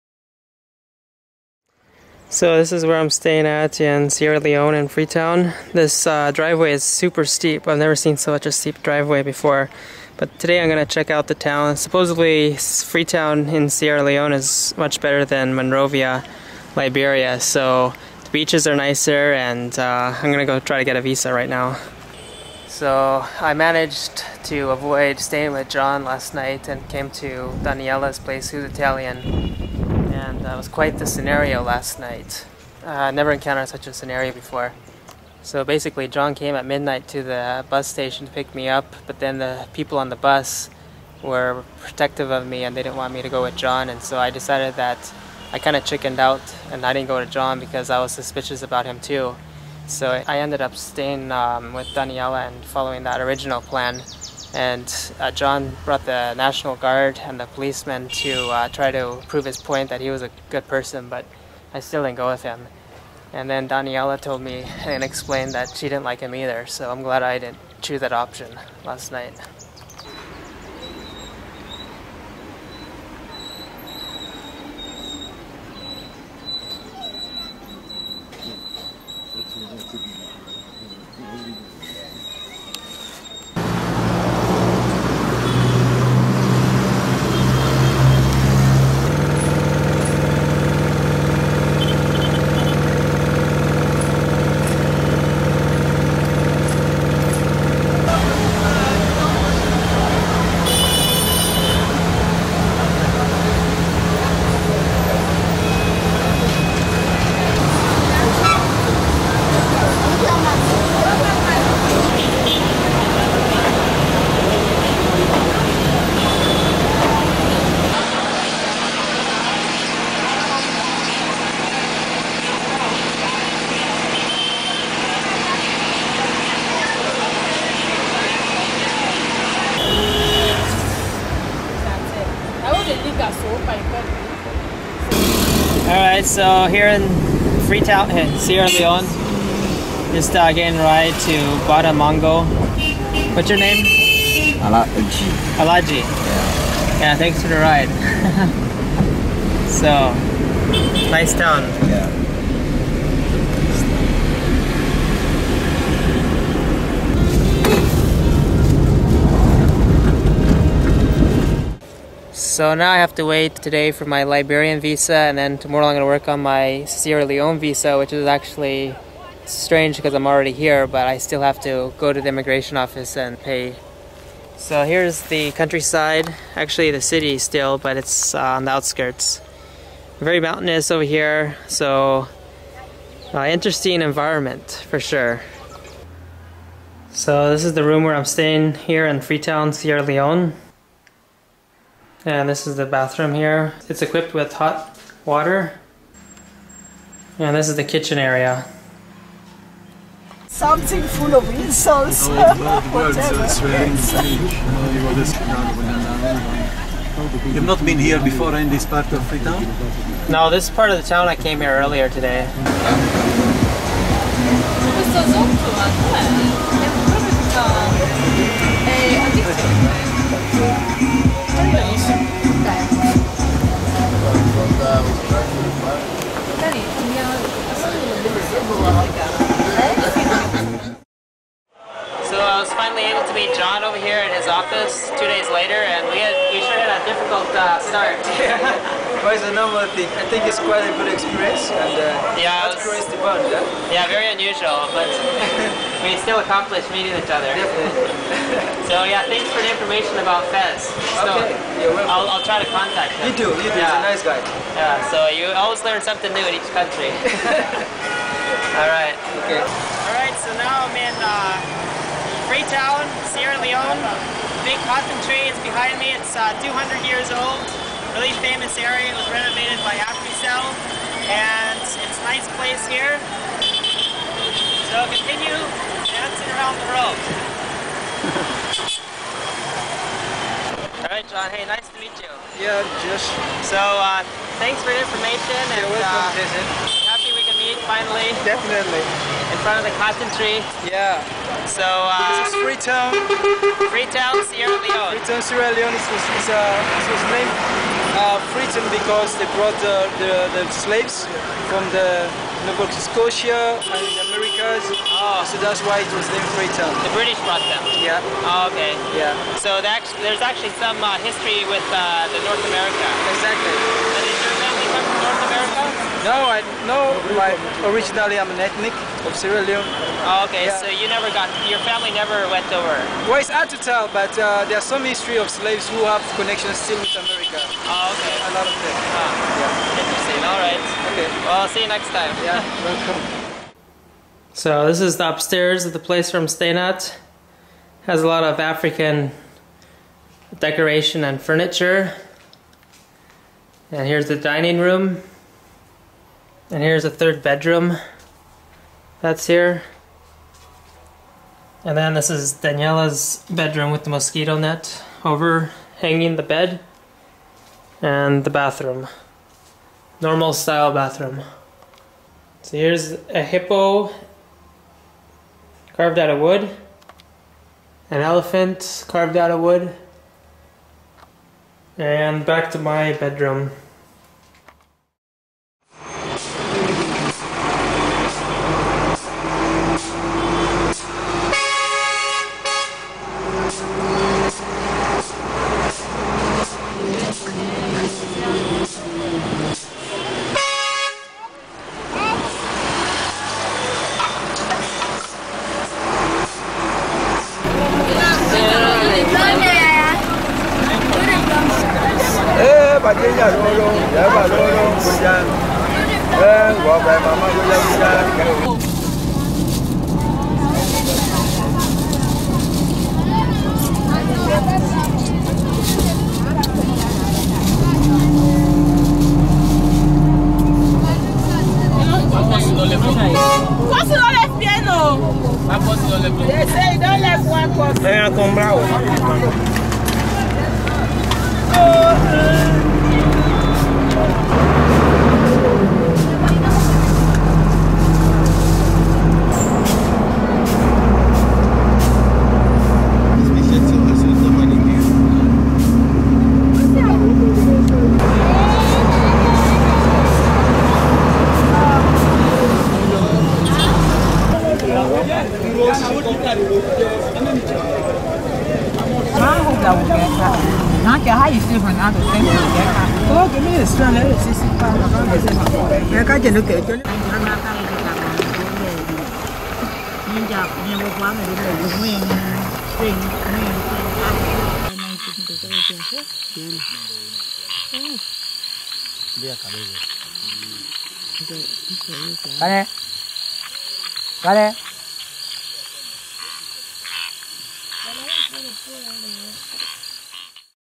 Freetown, Sierra Leone and taking the bus to Kadema then Liberia
Nov.38-30, 2012